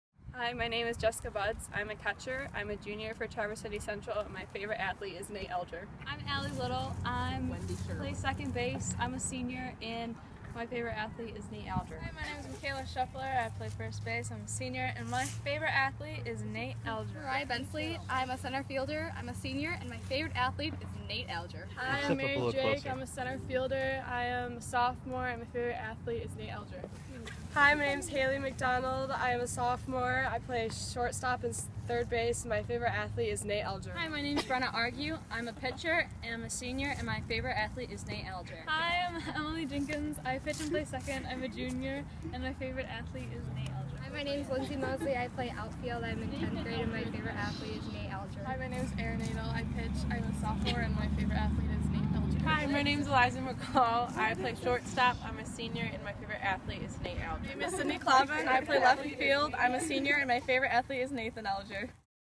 2015 TC Central Varsity Softball team
Meet the athletes of the 2015 Traverse City Central High School varsity softball team.